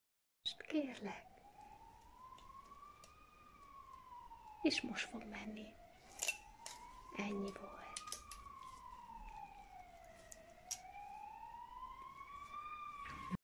Férfi füllyukasztás, fülbelövés Studex System75 füllyukasztó rendszerrel - Százszorszép kozmetika
Százszorszépes Szépségem!

Ki mondta, hogy a füllyukasztás csakis a nők kiváltsága lehet? ... Egy jól megválasztott ékszerrel igenis erősítheti a vagány férfiasságot.

Levim választása a Studex®️ neon sorozatának🖤 éjfeketéjére 🖤esett...